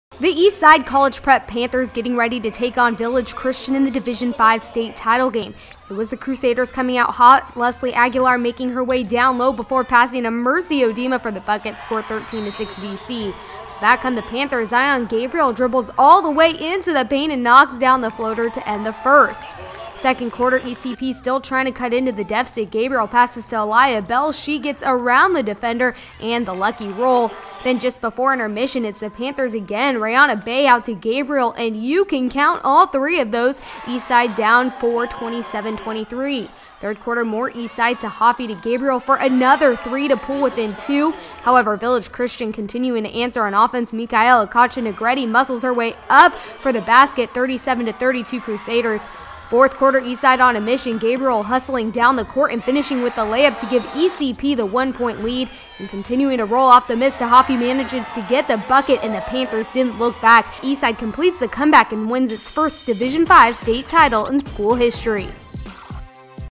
Eastside College Prep vs Village Christian Girls Hoops - 2016 CIF State Championship
THE EASTSIDE COLLEGE PREP PANTHERS GETTING READY TO TAKE ON VILLAGE CHRISTIAN IN THE DIVISION 5 STATE TITLE GAME

IT WAS THE CRUSADERS COMING OUT HOT, LESLIE AGUILAR MAKING HER WAY DOWN LOW BEFORE PASSING TO MERCY ODIMA FOR THE BUCKET SCORE 13 TO 6 VC

BUT BACK COME THE PANTHERS, ZION GABERIEL DRIBBLES ALL THE WAY INTO THE PAINT AND KNOCKS DOWN THE FLOATER TO END THE FIRST

SECOND QUARTER, ECP STILL TRYING TO CUT INTO THE DEFECIT, GABERIEL PASSES TO ALAYAH BELL, SHE GETS AROUND THE DEFENDER AND THE LUCKY ROLL

THEN JUST BEFORE INTERMISSION ITS THE PANTHERS AGAIN, RA'ANNA BAY OUT TO GABERIEL AND YOU CAN COUNT ALL THREE OF THOSE, EASTSIDE DOWN FOUR 27-23

THIRD QUARTER, MORE EASTSIDE, TAHAAFE TO GABERIEL FOR ANOTHER THREE TO PULL WITHIN TWO

HOWEVER, VILLAGE CHRISTIAN CONTINUING TO ANSWER ON OFFENSE MICAELA CACHO-NEGRETE MUSCLES HER WAY UP FOR THE BASKET, 37 TO 32 CRUSADERS

FOURTH QUARTER, EASTSIDE ON A MISSION, GABERIEL HUSTLING DOWN THE COURT AND FINISHING WITH THE LAY-UP TO GIVE ECP THE ONE POINT LEAD

AND CONTINUING TO ROLL, OFF THE MISS TAHAFEE MANAGES TO GET THE BUCKET AND THE PANTHERS DIDNT LOOK BACK EASTSIDE COMPLETES THE COMEBACK AND WINS ITS FIRST STATE DIVSION 5 TITLE IN SCHOOL HISTORY.